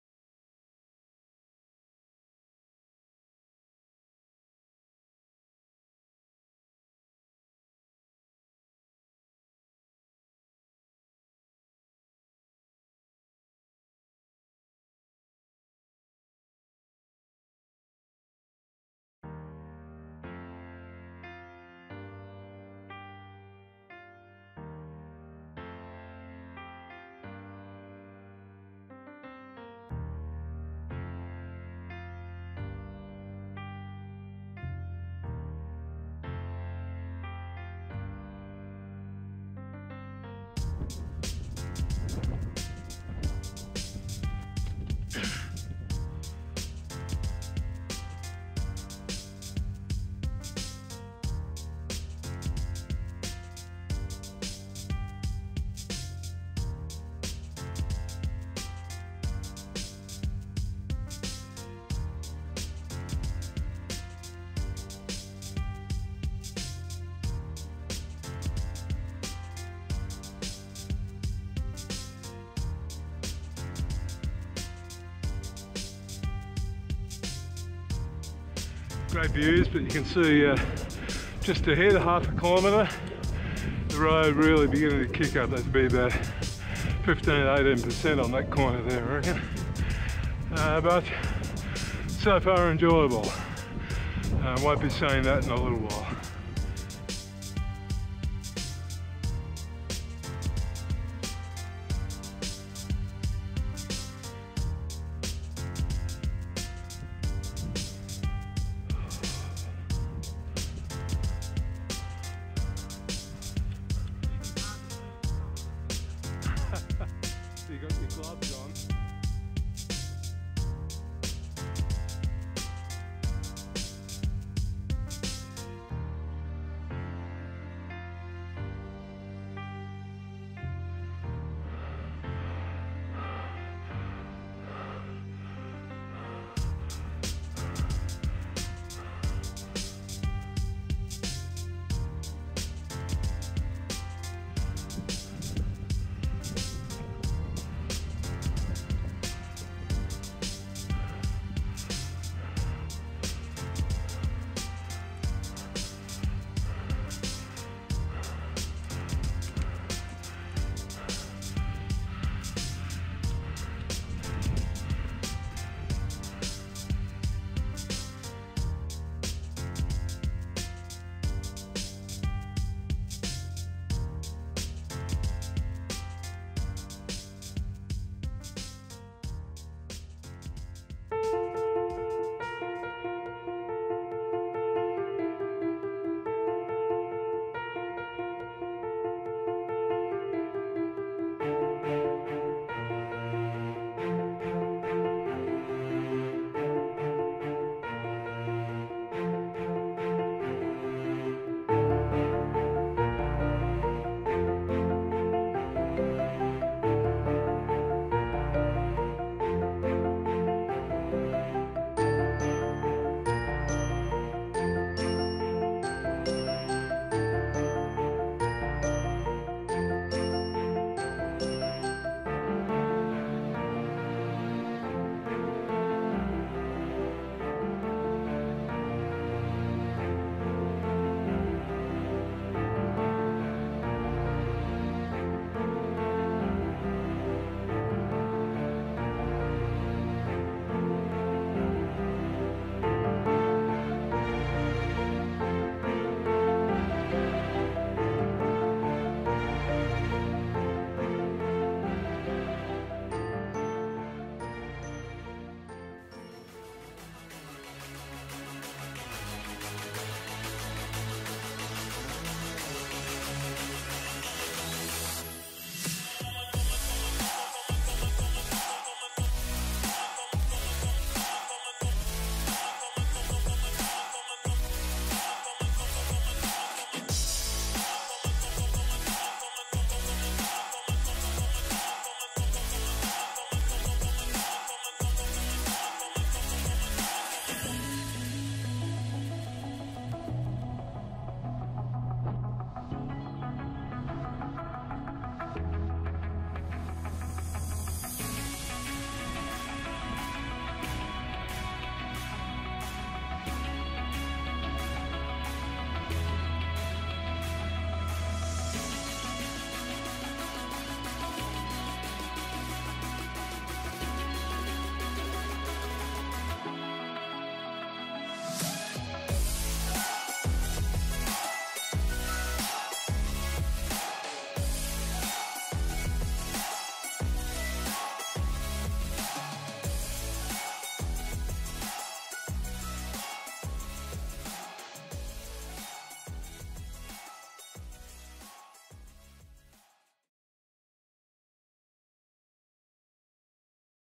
Cycling the Bealach an Ba (Applecross Pass) - Scotland UK
Short video of our crossing of the Bealach na Ba during our unsupported Scottish Highlands Cycle 2019. The stunning pass is only 9km's long but is one of the steepest road ascents in the UK